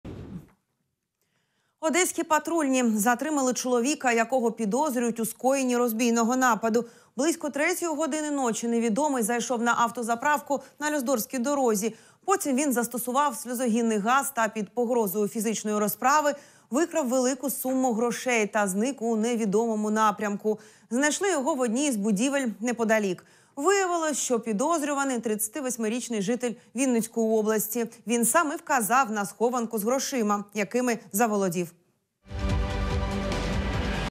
Одеські патрульні затримали чоловіка, якого підозрюють у скоєнні розбійного нападу
Близько третьої години ночі невідомий зайшов на автозаправку на Люстдорфській дорозі. Потім він застосував сльозогінний газ та під погрозою фізичної розправи викрав велику суму грошей і зник у невідомому напрямку.

Знайшли його в одній з будівель неподалік. Виявилося, що підозрюваний — 38-річний житель Вінницької області. Він сам і вказав на схованку з грошима, якими заволодів.